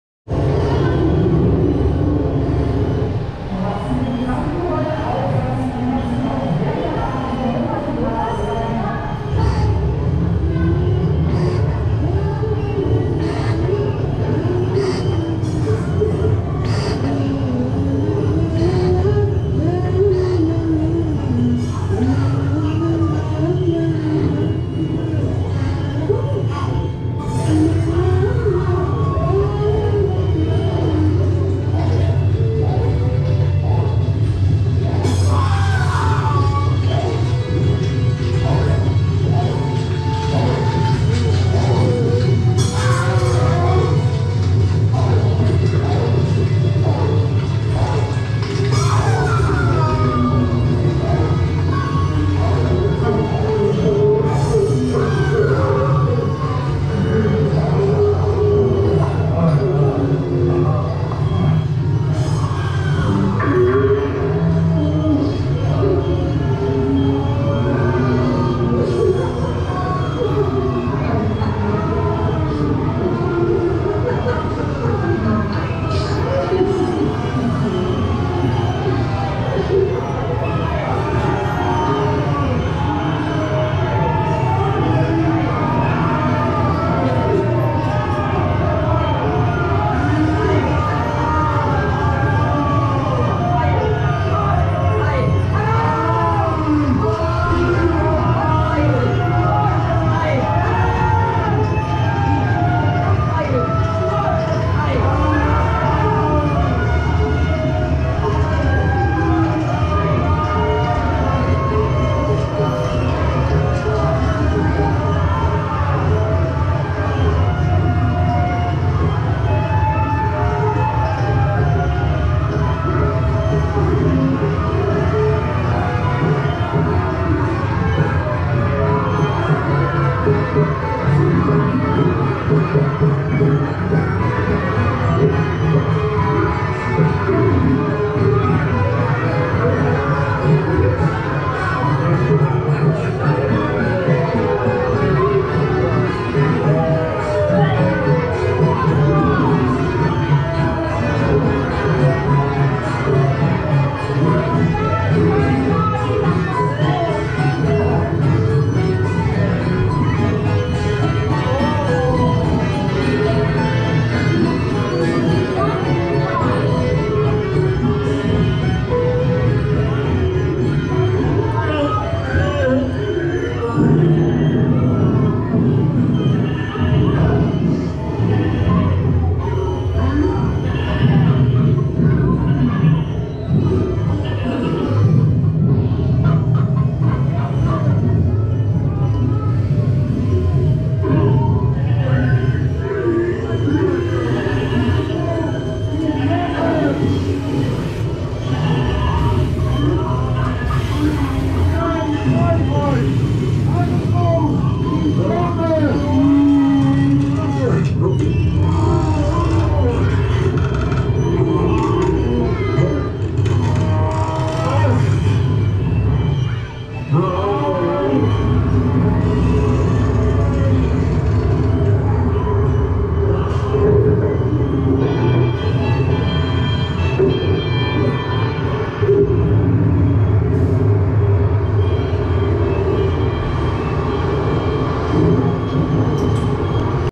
Burg Falkenstein POV, Holiday Park Mack Dark Ride (Emphasis on the Dark) | Non-Copyright
Burg Falkenstein is the single dark ride at Holiday Park in Germany.  This Mack haunted house features an omnimover ride system and some dark scenes with gore and torture.